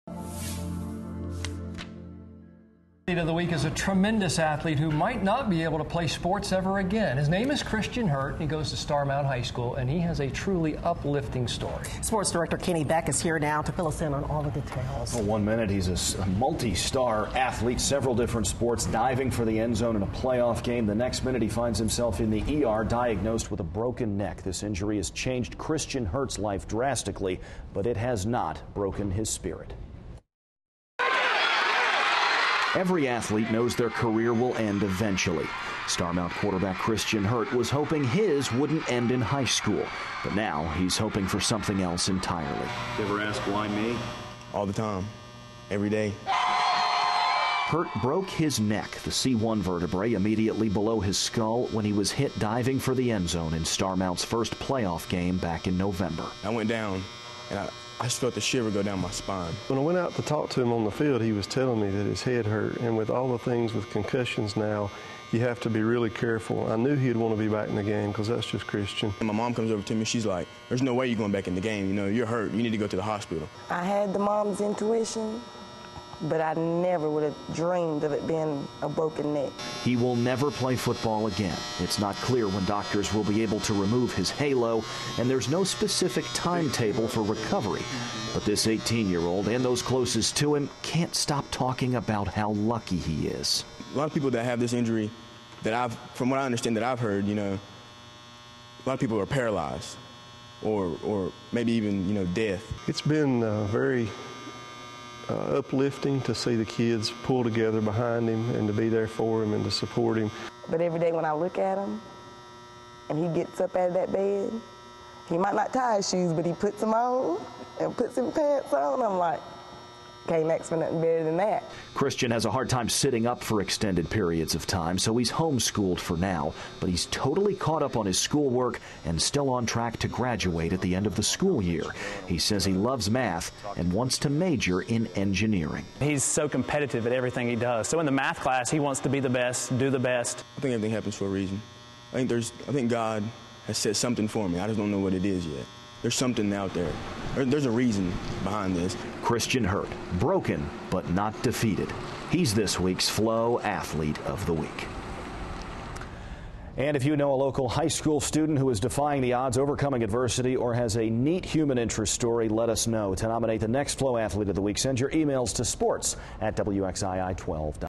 Athlete Of The Week: Starmount's Christian Hurt
A star quarterback that's dealing with not being able to play sports again but his spirit remains intact, meet Starmount's Christian Hurt.